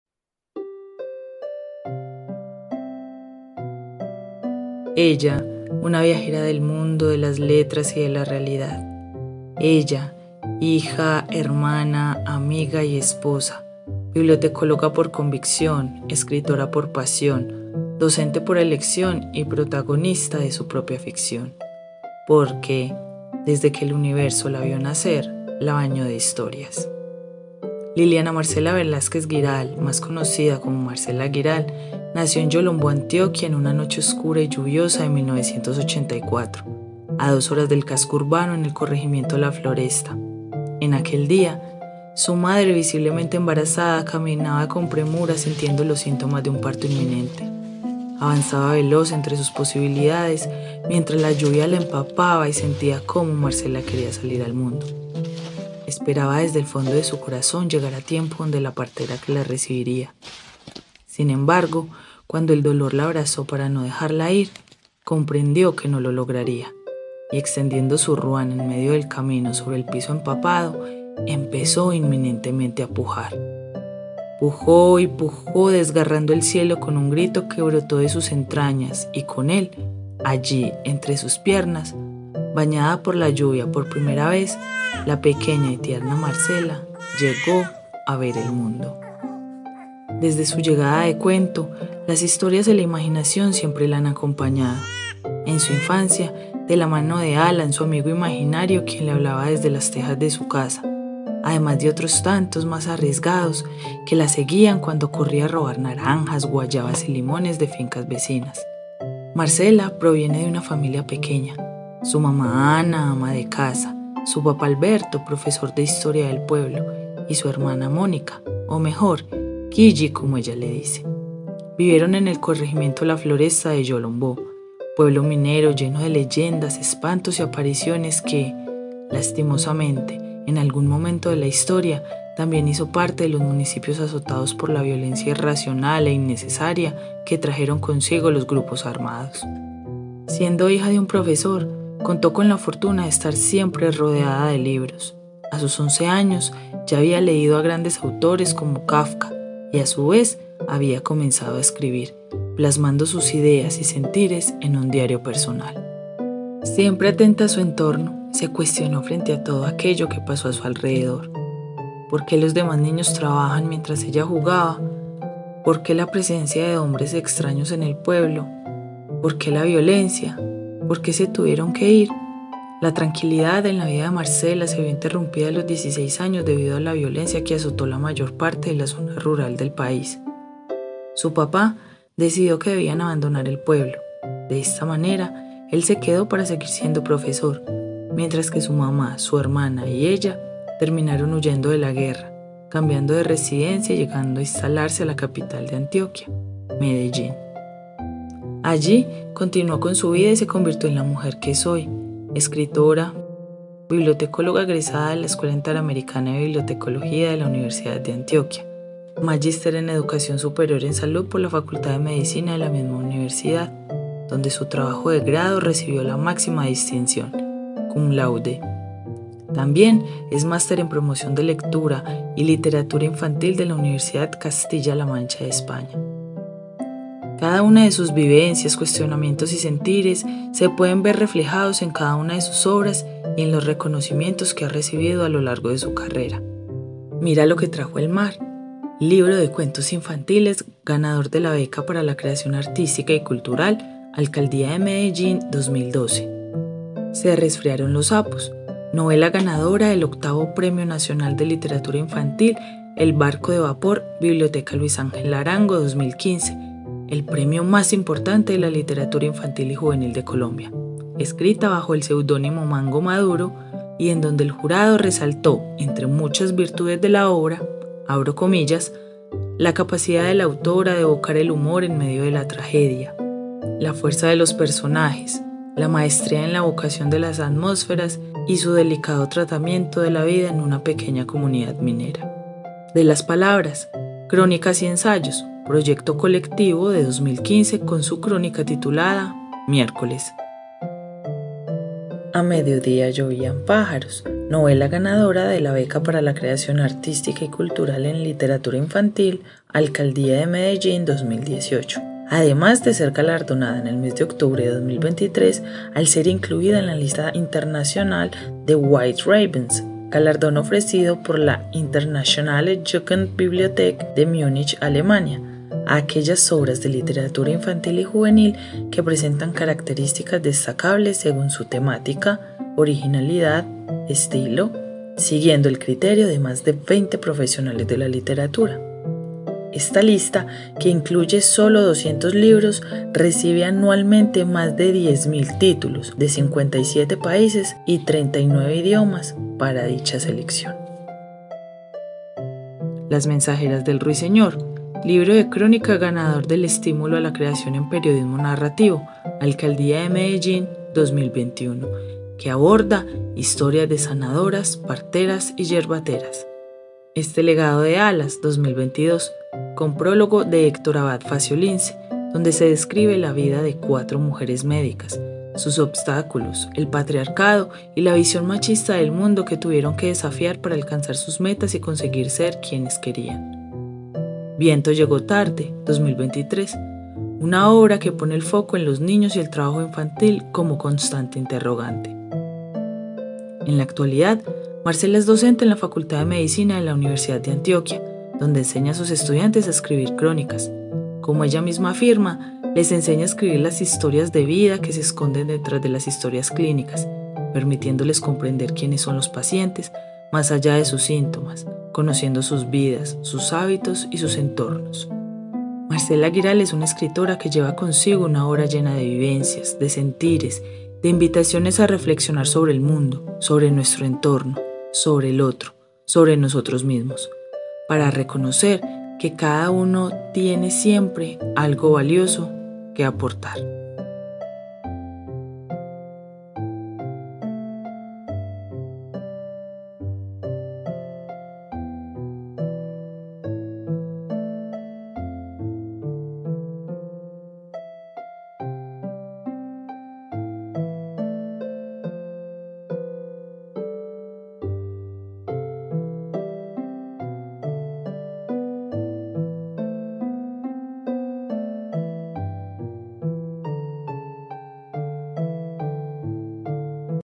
MARCELA GUIRAL | El autor y su obra | Breves apuntes
Ella, una viajera del mundo, de las letras y de la realidad. Ella, hija, hermana, amiga y esposa, bibliotecóloga por convicción, escritora por pasión, docente por elección y protagonista de su propia ficción, porque, desde que el universo la vio nacer, la bañó de historias.
Liliana Marcela Velásquez Guiral, más conocida como Marcela Guiral, nació en Yolombó, Antioquia, en una noche oscura y lluviosa de 1984, a dos horas del casco urbano, en el corregimiento La Floresta. En aquel día, su madre visiblemente embarazada caminaba con premura sintiendo los síntomas de un parto inminente. Avanzaba veloz entre sus posibilidades, mientras la lluvia la empapa y sentía como Marcela quería salir al mundo. Esperaba desde el fondo de su corazón llegar a tiempo donde la partera que la recibiría, sin embargo, cuando el dolor la abrazó para no dejarla ir, comprendió que no lo lograría y extendiendo su ruana en medio del camino, sobre el piso empapado, empezó inminentemente a pujar… pujó y pujó, desgarrando el cielo con un grito que brotó de sus entrañas y con él, allí, entre sus piernas, bañada por la lluvia por primera vez, la pequeña y tierna Marcela llegó a ver el mundo.